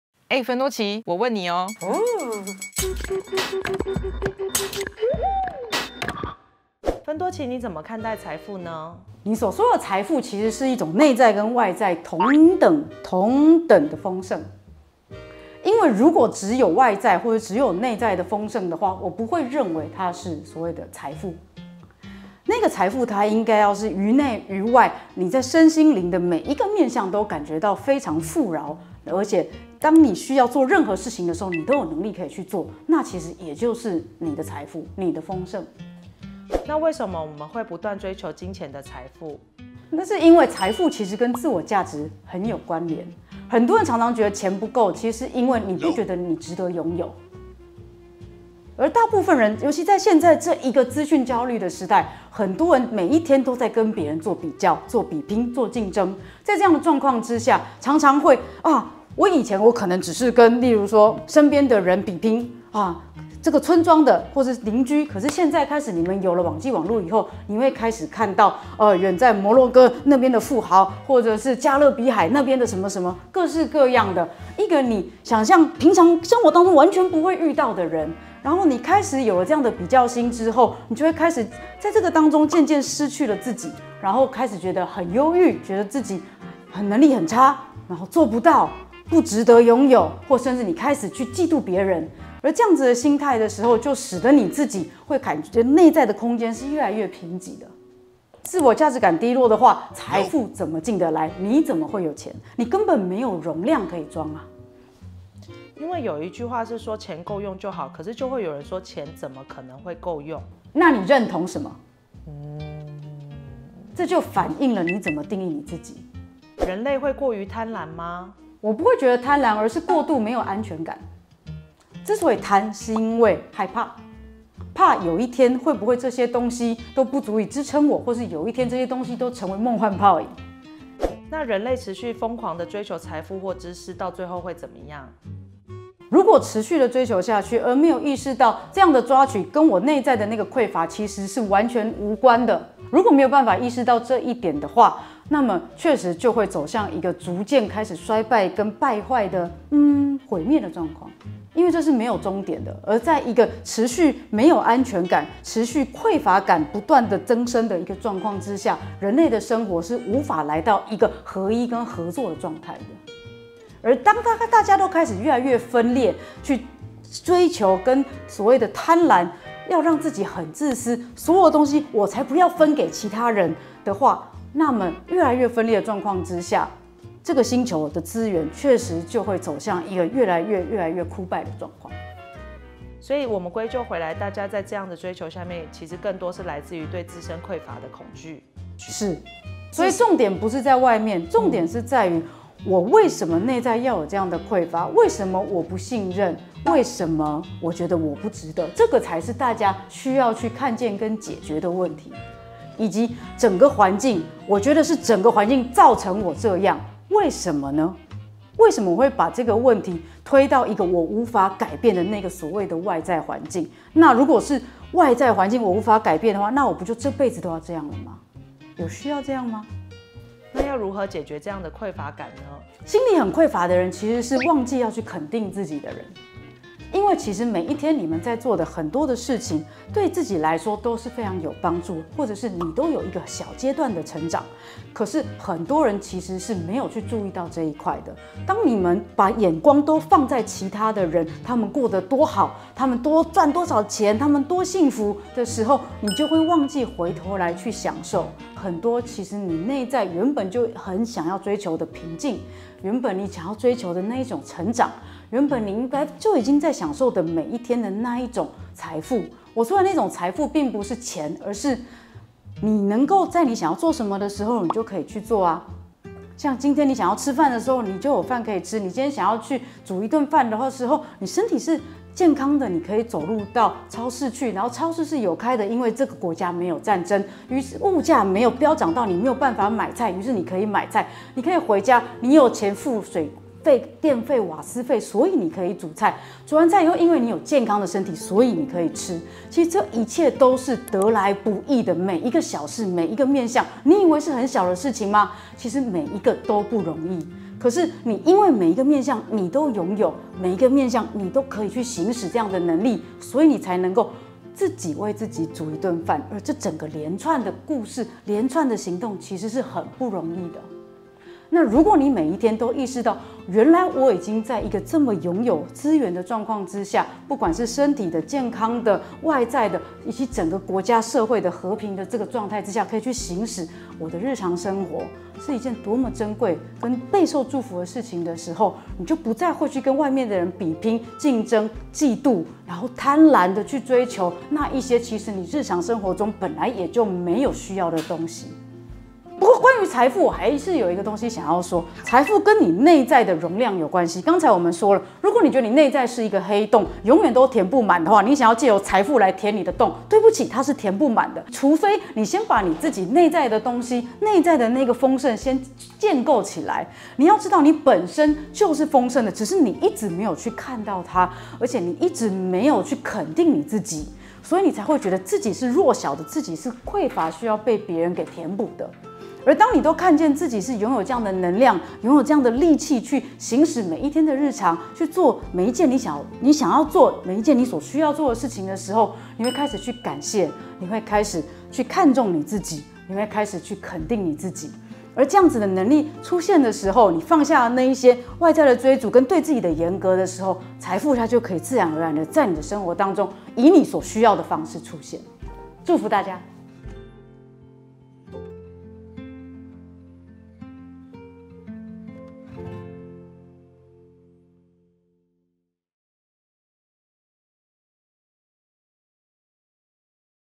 要如何才能擁有財富呢？〖分多奇Q&A〗
＊歡迎點開CC字幕觀看＊

在我們不斷的追求財富、一直向外看別人擁有什麼的時候，
常常都會忘記享受自己所擁有的，但其實這一切都得來不易，
記得下次回頭看看自己吧～